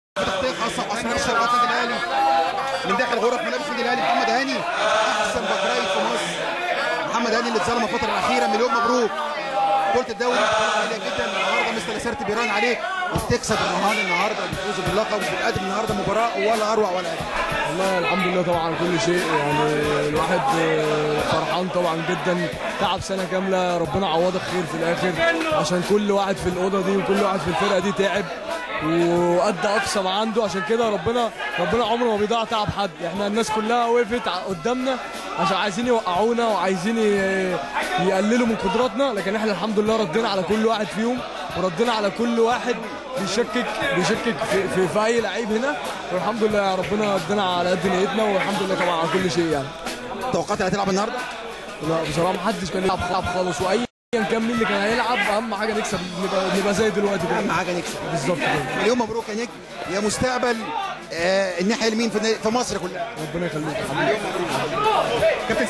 محمد هانى : تحقيق بطولة الدورى "41" هو رد على كل واحد شكك فى قدرات لاعبى الاهلى
التردد: 12341 أفقي.
القمر: نايل سات.
معامل الخطأ: 5/6.
معدل الترميز: 27500